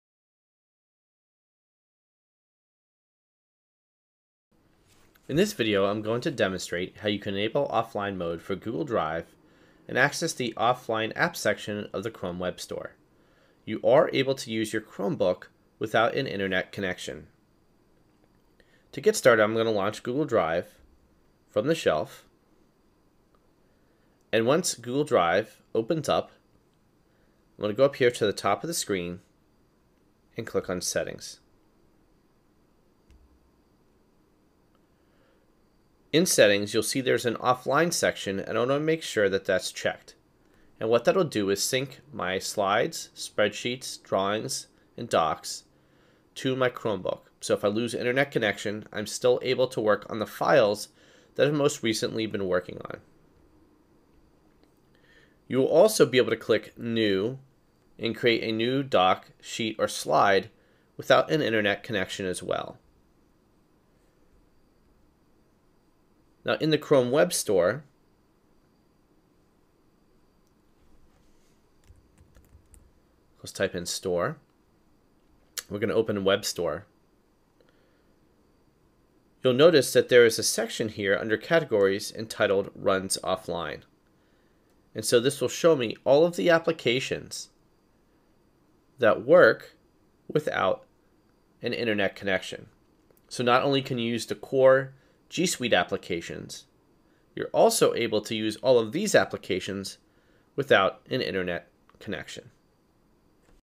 Chromebooks: Offline Apps
How to enable offline mode for Google Drive and view the collection of offline apps in the Chrome Web Store.